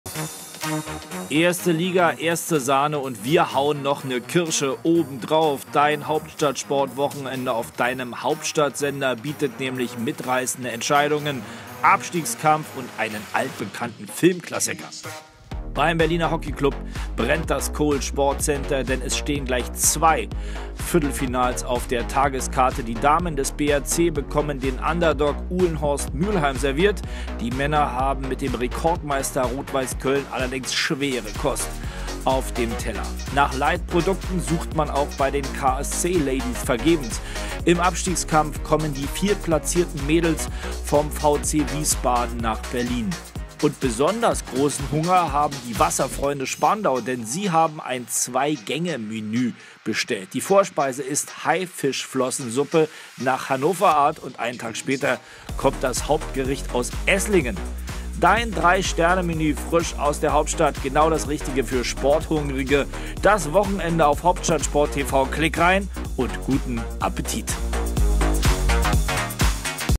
[Spoileralarm] Sportmenü
Es wird geguckt was auf den Tisch kommt und das ist diesmal wieder besonders Schmackhaft! Ein großer Sportlerteller mit dreierlei Disziplinen, Volleyball, Hockey und Wasserball! Guten Appetit!